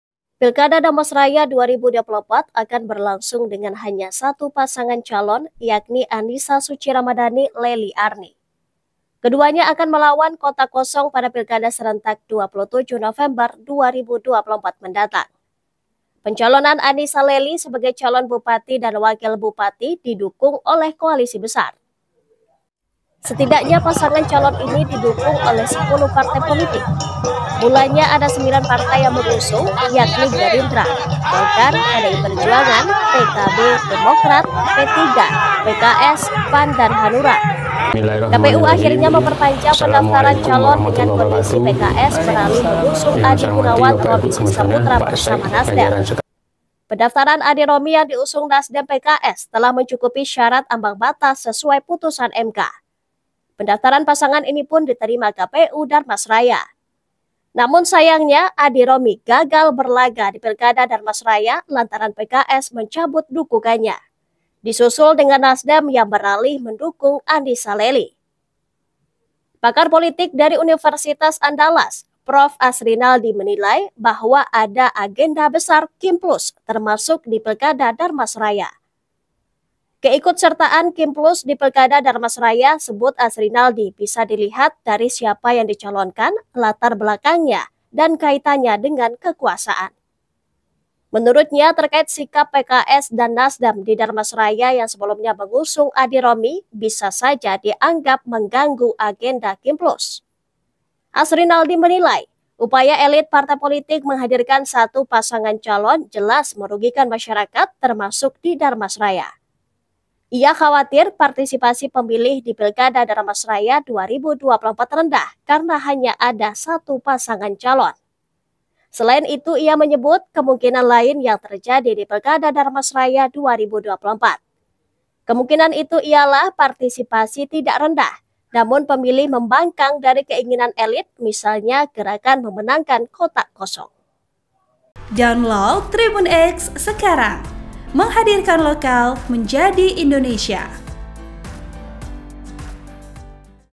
Calon Tunggal di Pilkada Dharmasraya, Pakar Politik Asrinaldi Singgung Agenda KIM Plus
TRIBUN-VIDEO.COM - Manajemen Semen Padang FC sepakat untuk mendatangkan pelatih asing menggantikan Hendri Susilo.

Pilkada Dharmasraya 2024 hanya akan berlangsung dengan satu pasangan calon, yakni Annisa Suci Ramadhani - Leli Arni. Keduanya akan melawan kotak kosong pada Pilkada serentak 27 November 2024 mendatang. Pencalonan Annisa - Leli sebagai calon bupati dan wakil bupati didukung oleh keluarga besar. Setidaknya pasangan calon ini didukung oleh 10 partai politik. Mulanya ada sembilan partai yang mengusung, yakni Gerindra, Golkar, PDI Perjuangan, PKB, Demokrat, PPP, PKS, PAN, dan Hanura. KPU akhirnya memperpanjang pendaftaran calon dengan kondisi PKS beralih mengusung Adi Gunawan - Romi Siska Putra bersama NasDem. Pendaftaran Adi - Romi yang diusung NasDem dan PKS telah memenuhi syarat ambang batas sesuai putusan MK, yakni 10 persen. Pendaftaran pasangan ini pun diterima KPU Dharmasraya. Namun sayang, Adi – Romi gagal berlaga di Pilkada Dharmasraya karena PKS mencabut dukungannya. Disusul dengan NasDem yang beralih mendukung Annisa - Leli. Pakar politik dari Universitas Andalas Prof Asrinaldi menilai bahwa ada agenda besar Koalisi Indonesia Maju (KIM) Plus, termasuk di Pilkada Dharmasraya. Keikutsertaan KIM Plus di Pilkada Dharmasraya sebut Asrinaldi bisa dilihat dari siapa yang dicalonkan, latar belakang dan pengaruhnya dengan kekuasaan. Menurutnya, terkait sikap PKS dan NasDem di Dharmasraya yang sebelumnya mengusung Adi - Romi bisa saja dianggap mengganggu agenda KIM Plus. Asrinaldi menilai, upaya partai elit politik menghadirkan satu pasangan calon jelas merugikan masyarakat, termasuk di Dharmasraya. Ia khawatir partisipasi pemilih di Pilkada Dharmasraya 2024 rendah karena hanya ada satu pasangan calon. Selain itu, ia menyebut kemungkinan lain yang terjadi di Pilkada Dharmasraya 2024. Kemungkinan itu adalah partisipasi tidak rendah, namun pemilih membangkang dari keinginan elit, misalnya gerakan memenangkan kotak kosong.